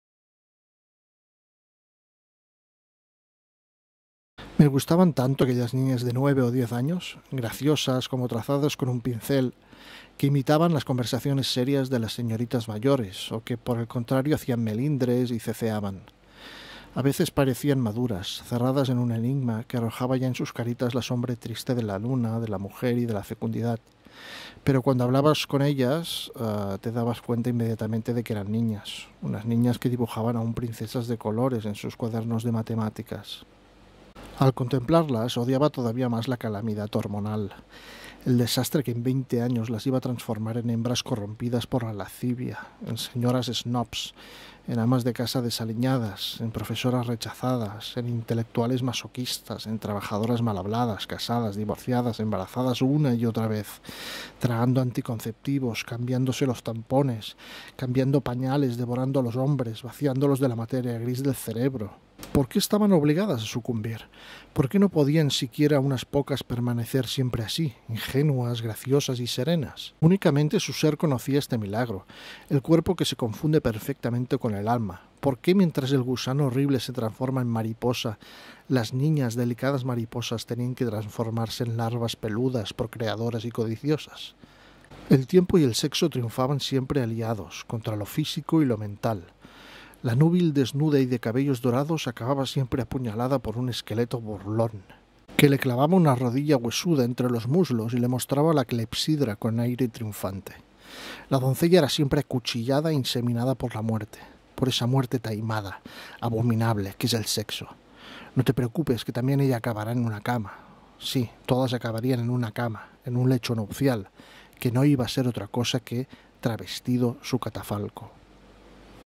De niñas a mujeres por Mircea Cărtărescu
Fragmento de "Lulu" de Mircea Cărtărescu.
Ilustraciones de Waldemar von Kozak.